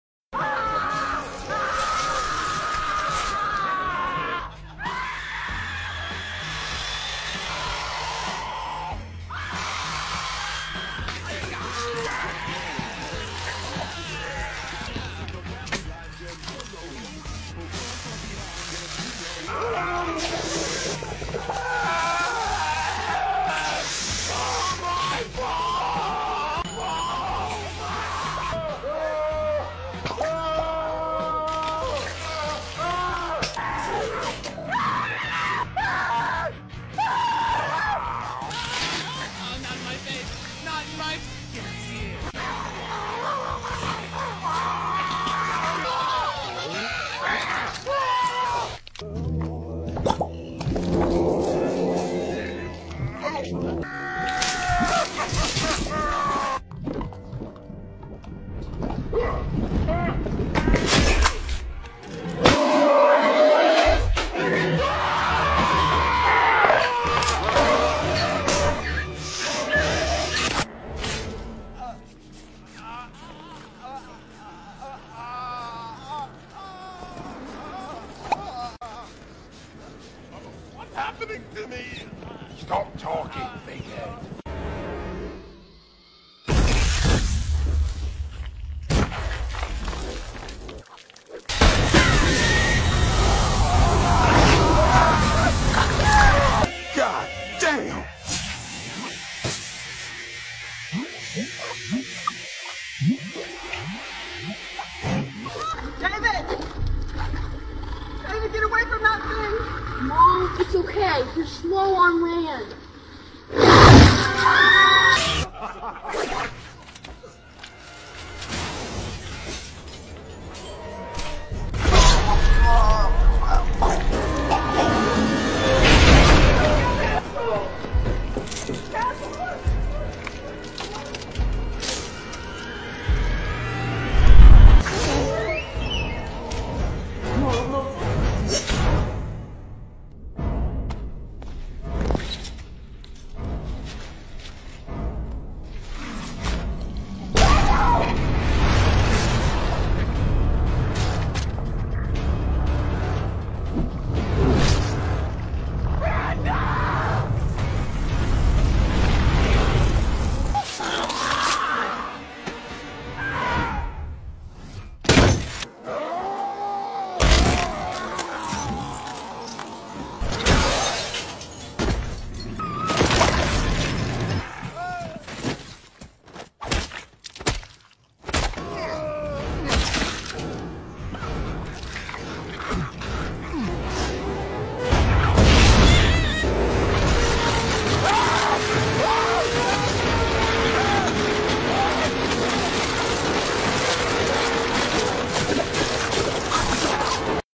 LES SCÈNES LES PLUS GORE DES FILMS D'HORREUR CULTES
Crédits : Je ne suis pas propriétaire du contenu de cette vidéo, c'est un Mix de scènes trouvées sur FILMS TRASH HORROR TERROR sur FACEBOOK.